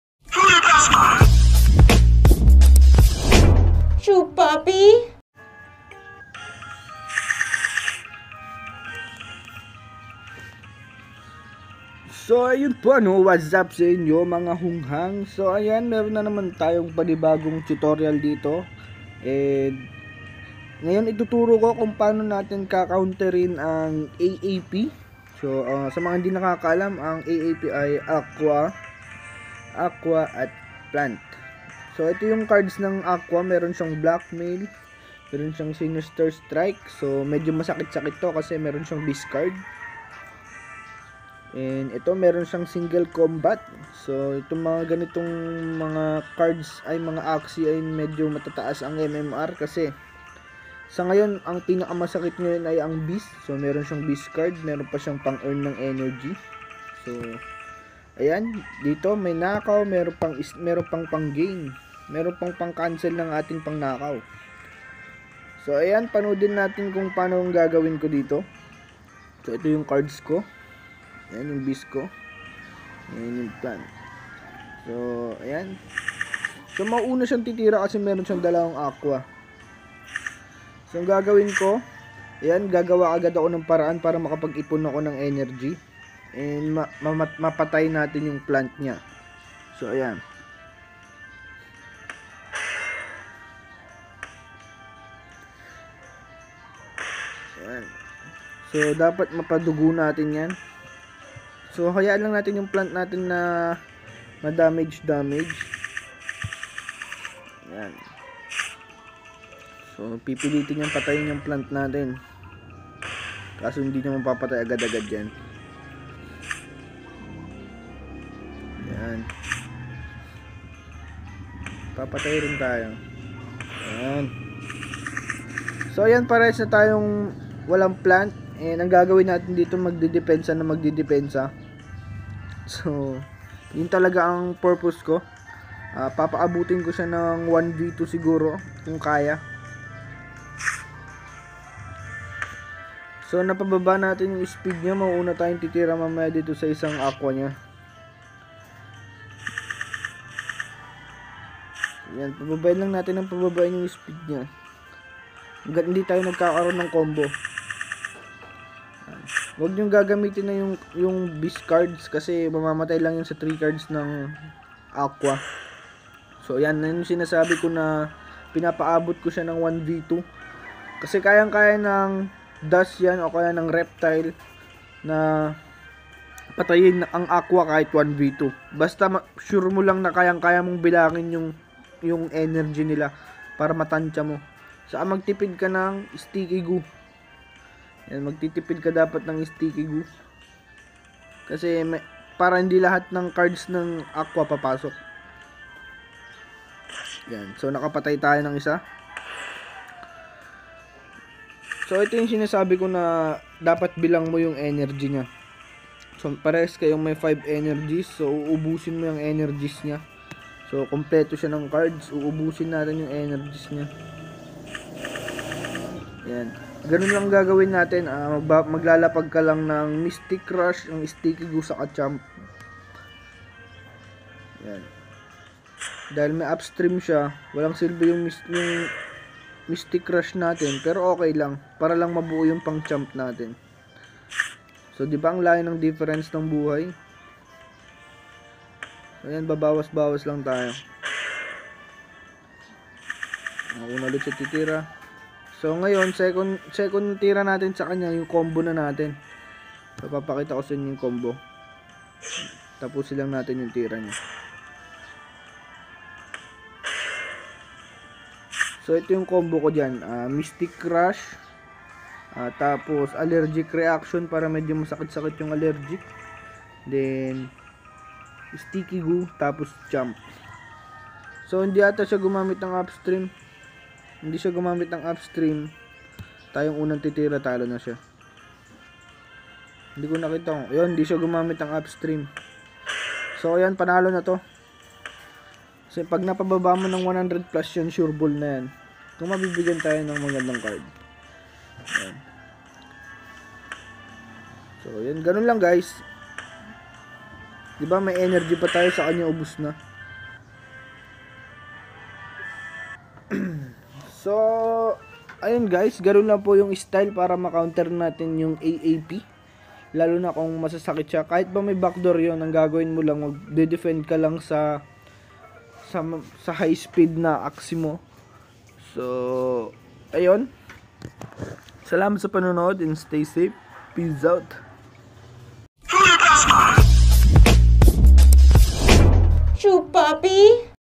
Counter AAP: Dusk + Beast + Plant Lineup (Dusk vs 2 Aqua) DBP/RBP Strategy
Counter AAP using Dusk, Beast and Plant Lineup